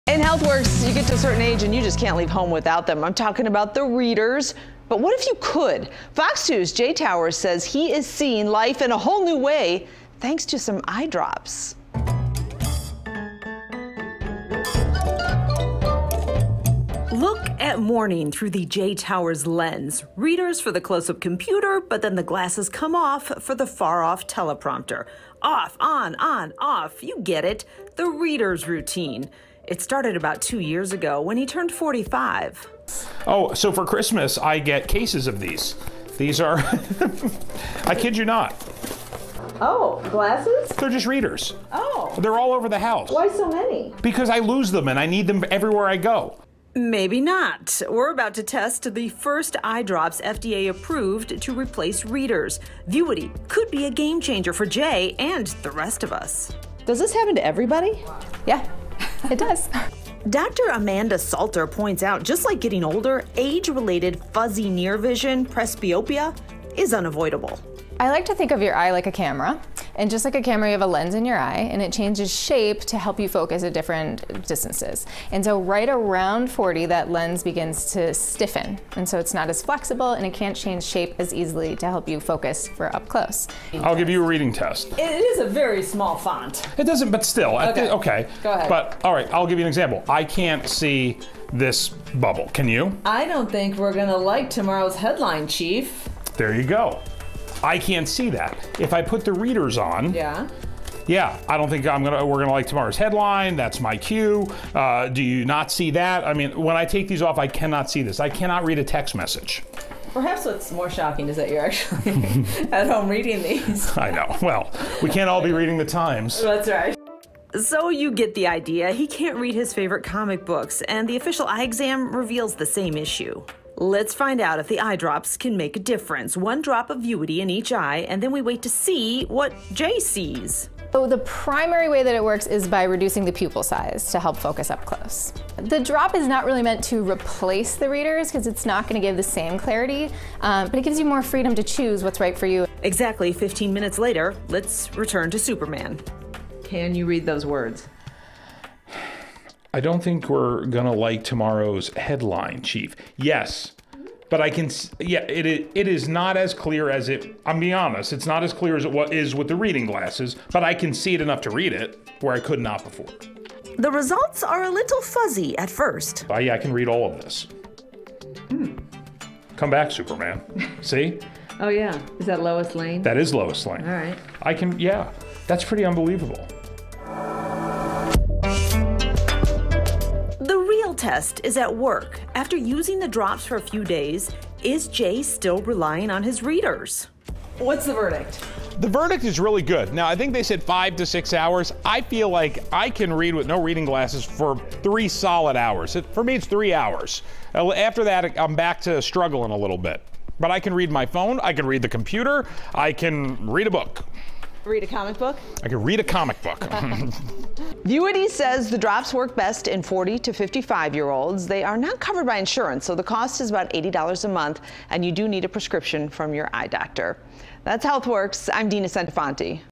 Can prescription eye drops replace reader glasses? Jay Towers finds out
When you reach a certain age, many people can't leave home without readers. but now there's a new prescription eye drop that promises to enable your aging eyes to read without glasses - could that work? We put the drops to test with FOX 2's Jay Towers